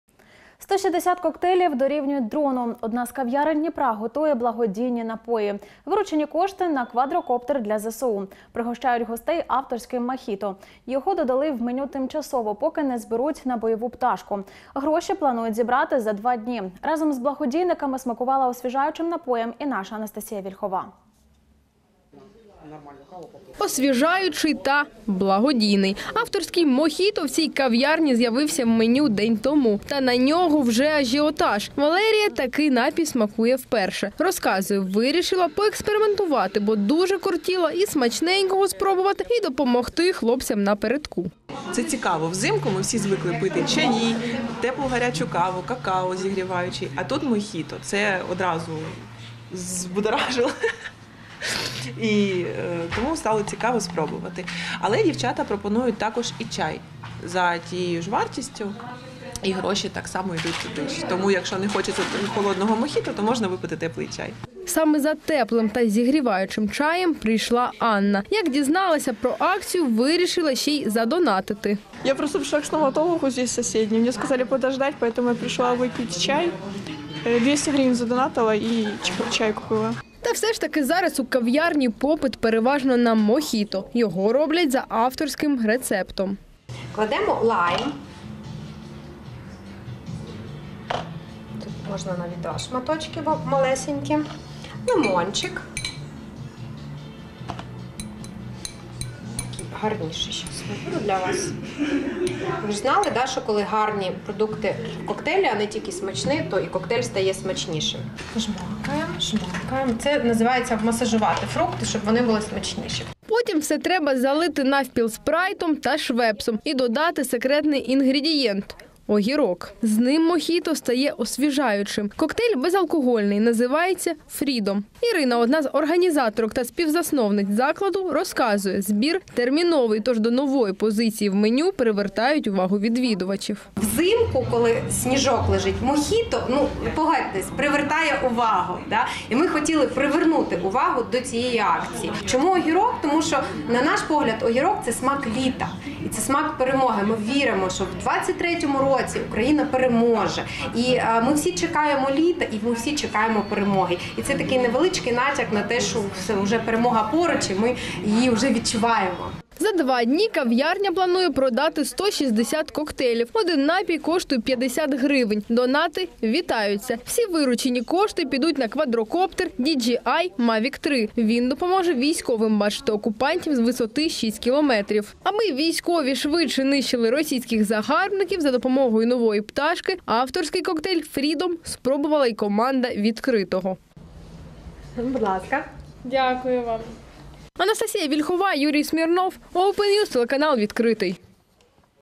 Коктейль на мавік: в кав‘ярні Дніпра збирать кошти на ЗСУ
У Дніпрі в кав'ярні продають коктейлі і збирають на дрон для ЗСУ. Де можна посмакувати і допомогти захисникам, дивіться в нашому сюжеті.